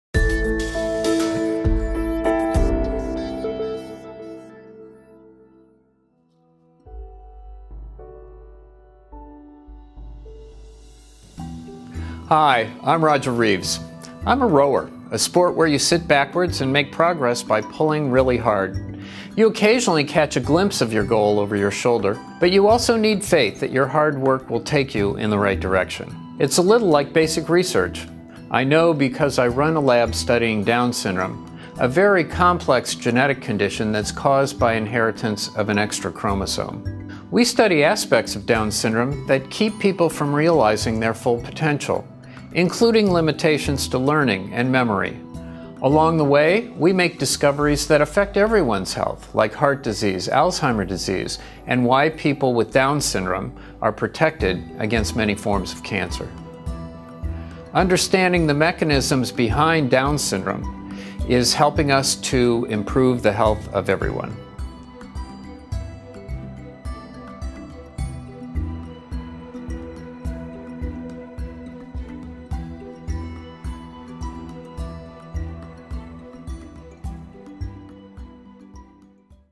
TomorrowsDiscoveries: Studying Down Syndrome for Everyone’s Benefit — Dr. Roger Reeves
TomorrowsDiscoveries: Research on Down syndrome can improve understanding of heart disease, Alzheimer’s and cancer. Dr. Roger Reeves and his team at Johns Hopkins study Down syndrome to improve general health.